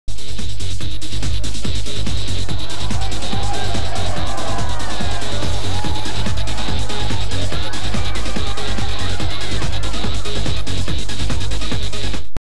Fox Footy Live Jr Football Bumper 2002
Fox Footy. Found on a 'Round 4 NM vs SYD' tape.

Head over to Batprince's game channel:
Youtube.com/Batprince
OR
Head over to Pete's Twitch
Twitch.tv/Peterdea